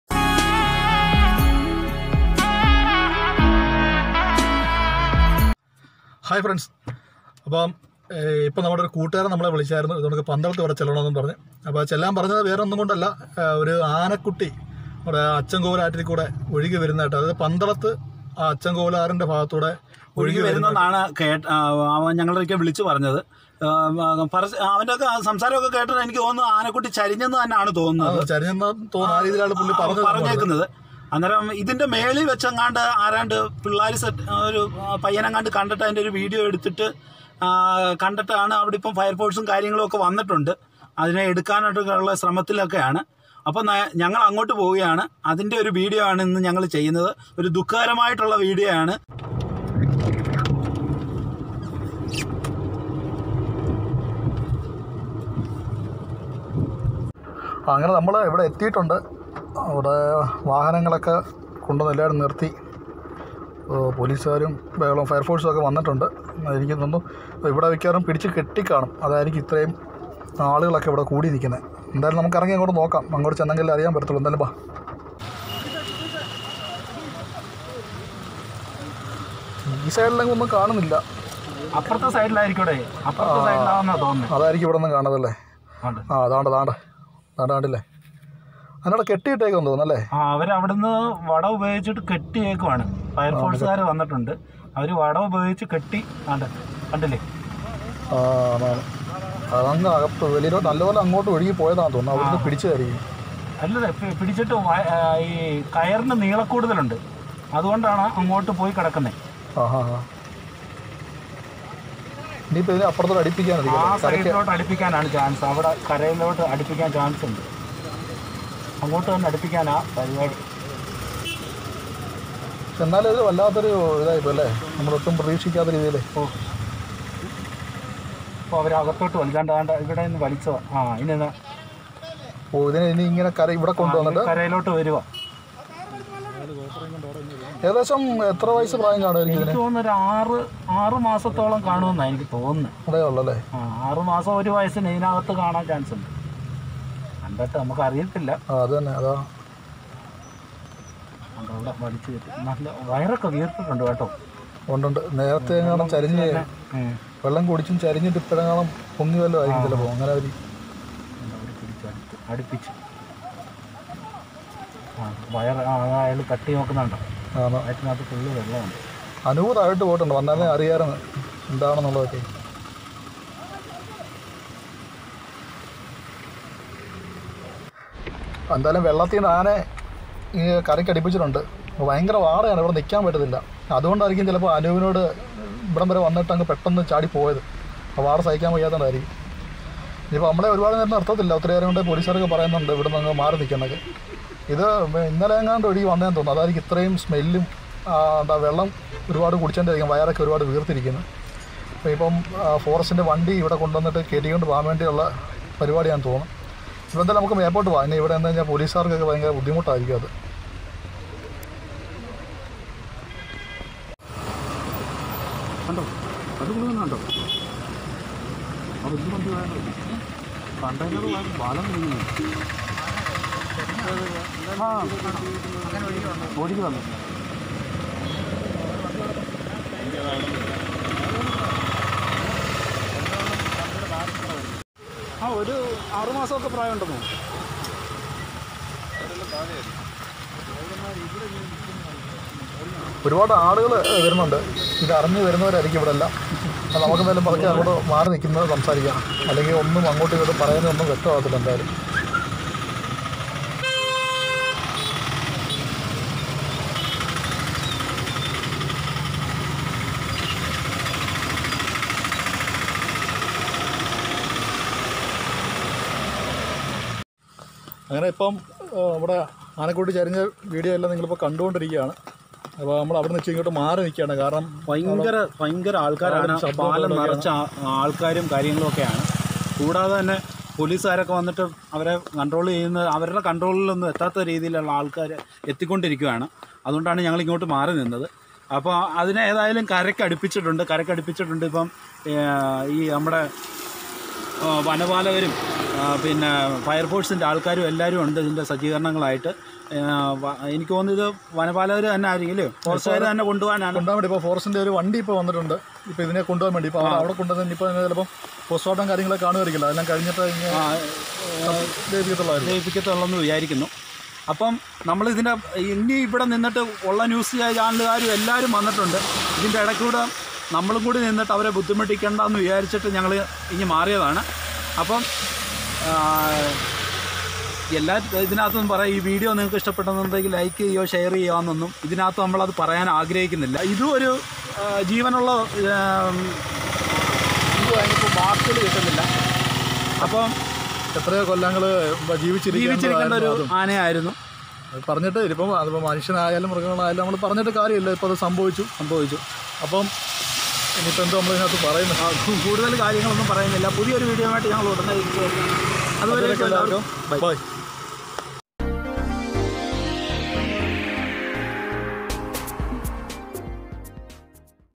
Elephent | കുത്തൊഴുക്കിൽ പെട്ട് അച്ചൻകോവിലാറിലൂടെ ഒരു ആനക്കുട്ടി
ആനക്കുട്ടി  ചരിഞ്ഞു. അച്ചൻകോവിലാറിൽ  ഒഴുകി വന്ന  ആനക്കുട്ടിയുടെ  ജഡം  അഗ്നിശമനസേനയും  കരയ്‌ക്കെടുത്തു